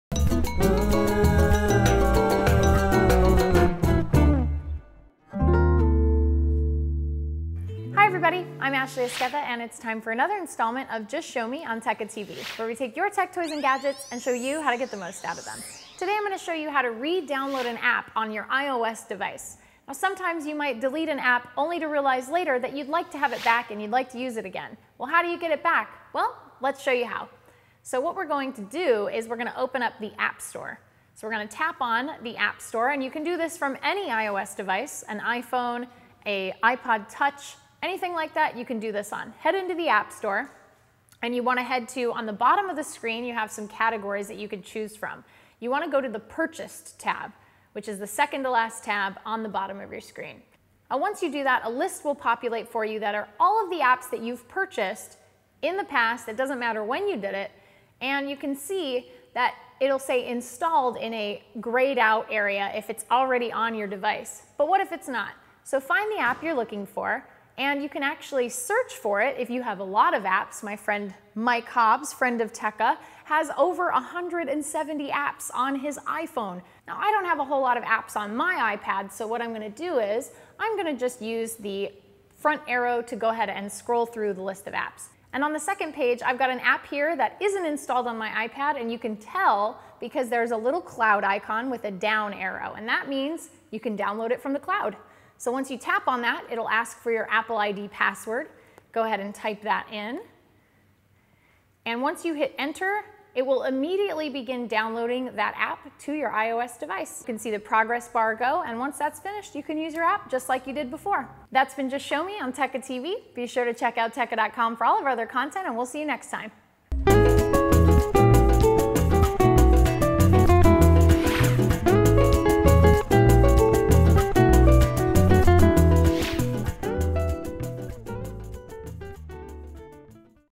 Just Show Me: How to redownload apps on an iPhone or iPad
In today's episode we'll show you how to redownload apps on your iPhone or iPad.

When you purchase an app from the iTunes store, you've purchased it for life (in 99.99% of cases). If you're not using the app anymore, it's okay to delete it from your iPad or iPhone; you can just download it again free of charge anytime you want.